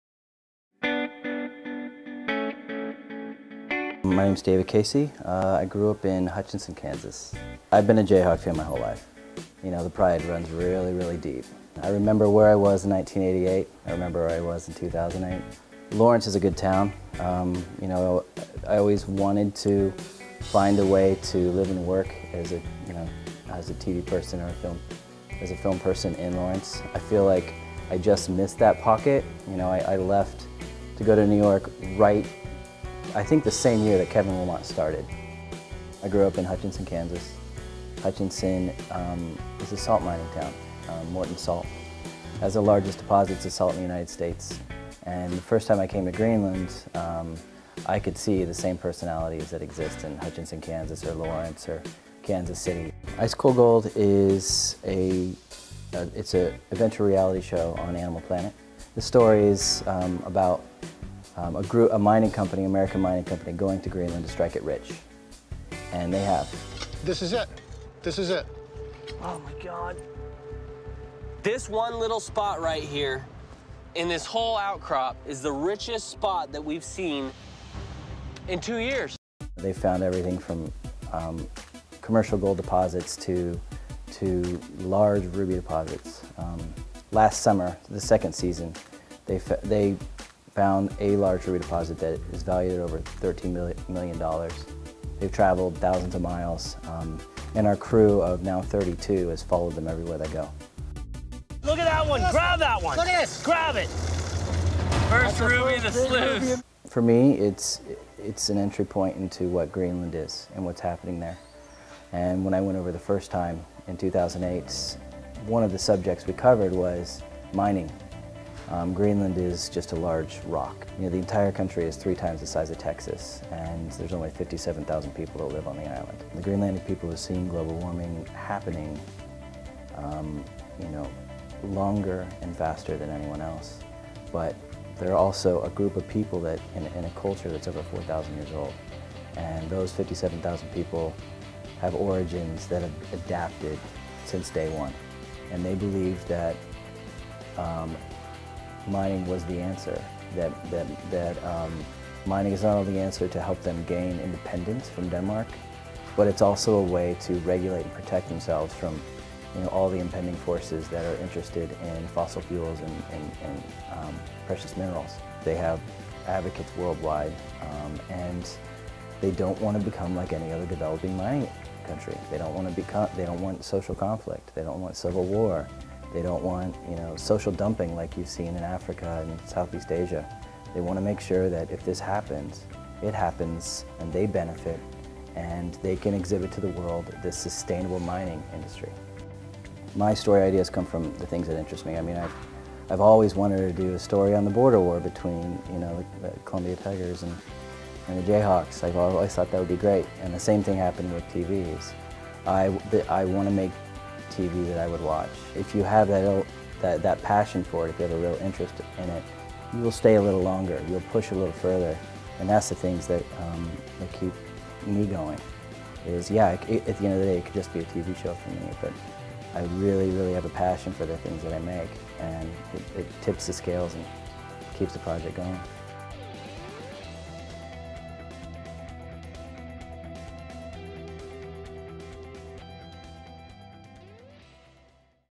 Television Producer, David Casey, Class of 2001
Television producer David Casey discusses his love for the Jayhawks and how he began a TV show 3,000 miles from Kansas: "Ice Cold Gold," an adventure series set in Greenland on the Animal Planet Network.